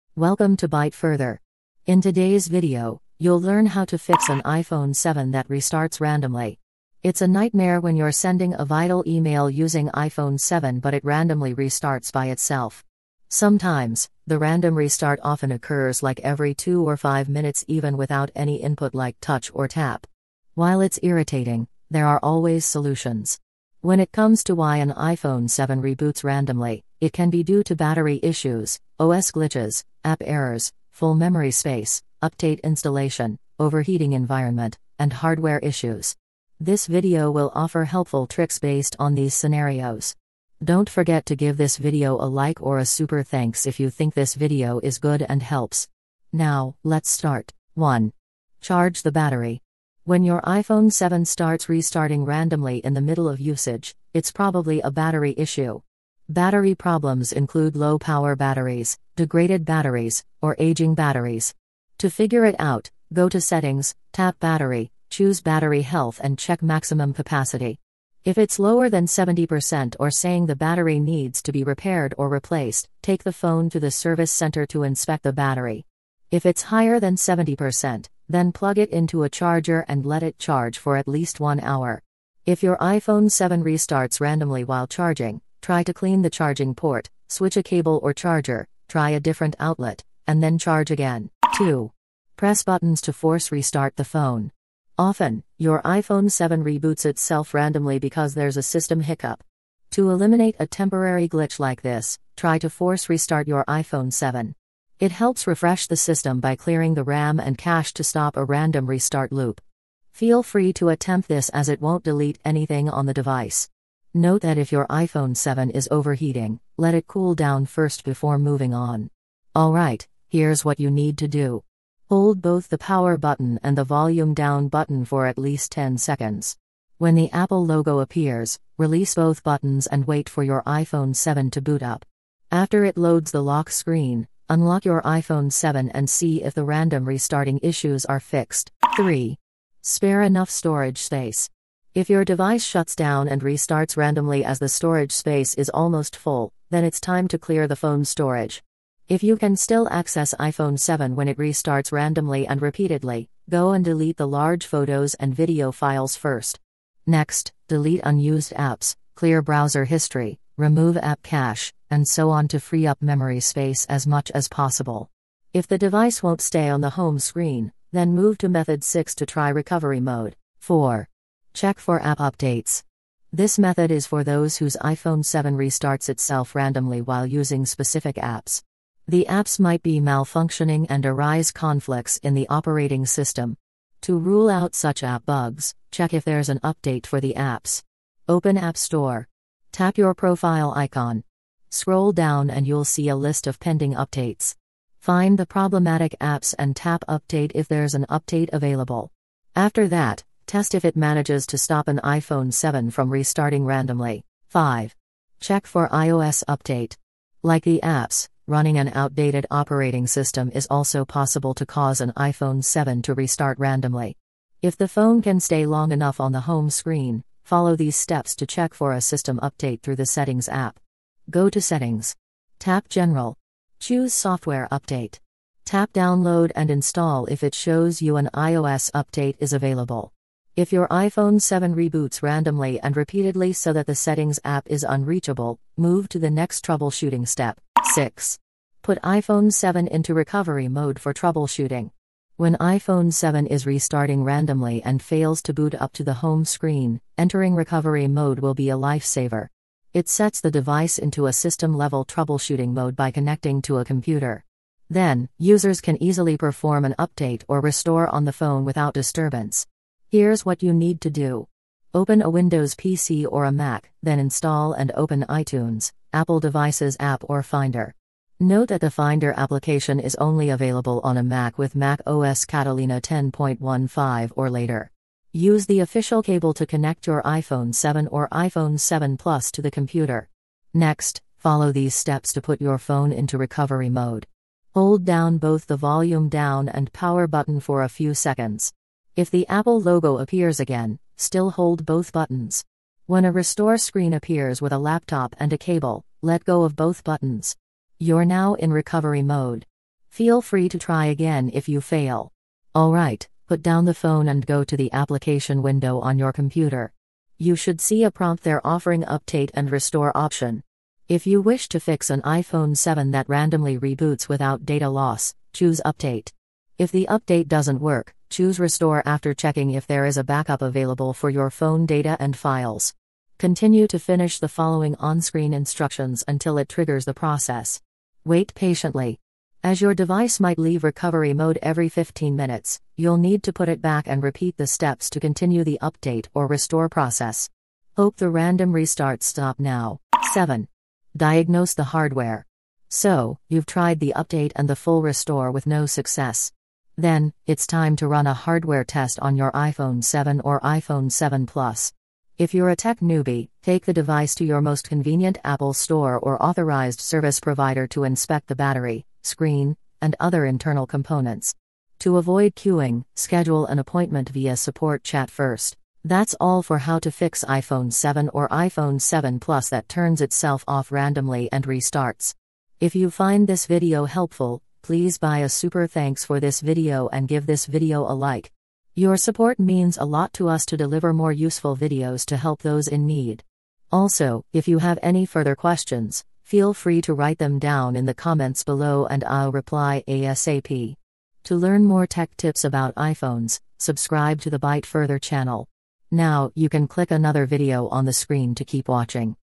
How to Fix an iPhone 7 (Plus) That Restarts Randomly By Itself: 7 Troubleshooting Steps
In today's video, you'll learn how to fix an iPhone 7 that restarts randomly. It's a nightmare when you're sending a vital email using iPhone 7 but it randomly restarts by itself. Sometimes, the random restart often occurs like every 2 or 5 minutes even without any input like touch or tap.
While it's irritating, there are always solutions. When it comes to why an iPhone 7 reboots randomly, it can be due to battery issues, OS glitches, app errors, full memory space, update installation, overheating environment, and hardware issues.

This video will offer helpful tricks based on these scenarios. Don't forget to give this video a like or a super thanks if you think this video is good and helps! Now, let's start.

On this video:

0:47 One. Charge the battery.
1:31 Two. Press buttons to force restart the phone.
2:18 Three. Spare enough storage space.
5:50 Four. Check for app updates.
3:24 Five. Check for iOS update.
3:57 Six. Put iPhone 7 into recovery mode for troubleshooting.
5:40 Seven. Diagnose the hardware.

Hope this video can help you fix an iPhone 7 (Plus) that restarts randomly by itself.